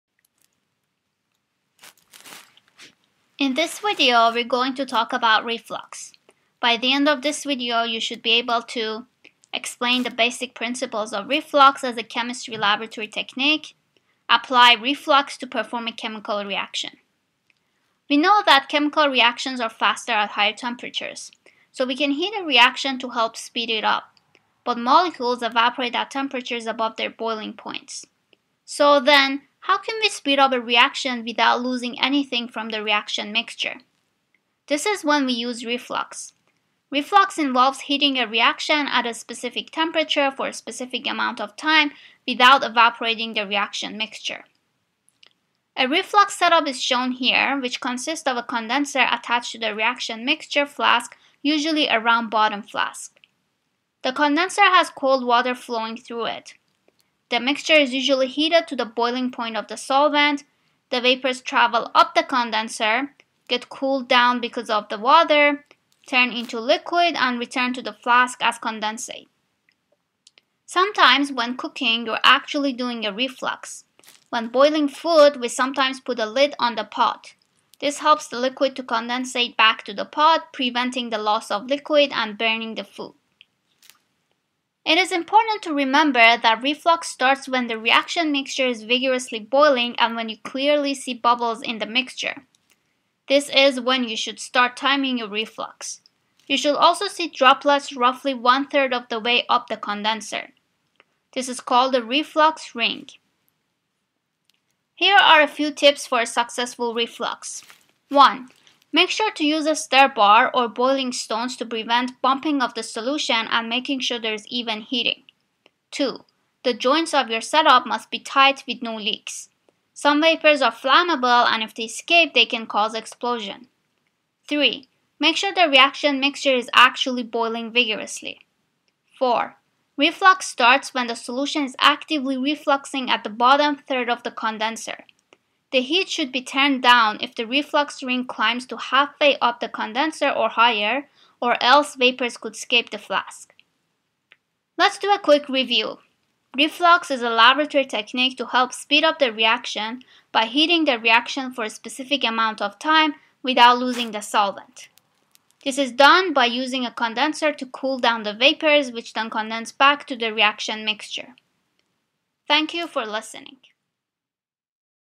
Reflux-Chemistry Laboratory Technique
This video explains the basic principles of reflux. Reflux is a method used to heat a reaction mixture for a specific amount of time at a specific temperature.